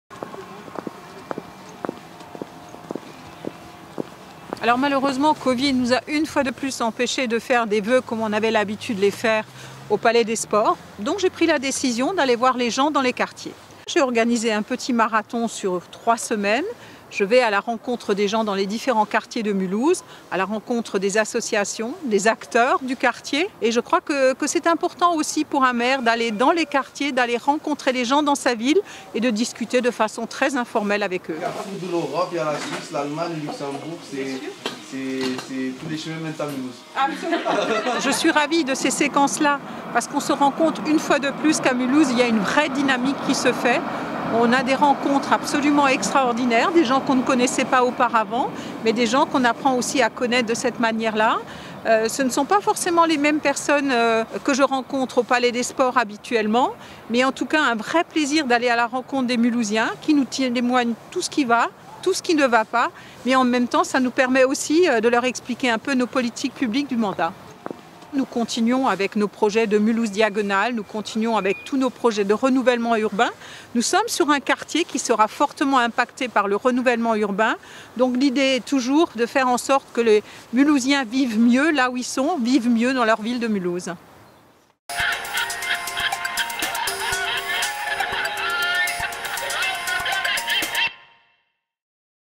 Vœux du maire dans les quartiers - reportage aux Coteaux
Crise sanitaire oblige, exit cette année la traditionnelle cérémonie des Vœux du maire de Mulhouse, au Palais des sports, et place aux vœux dans les quartiers. L’occasion pour Michèle Lutz de rencontrer les habitants et les forces vives aux quatre coins de la ville. Reportage sur le vif, signé M+, lors de la visite aux Coteaux.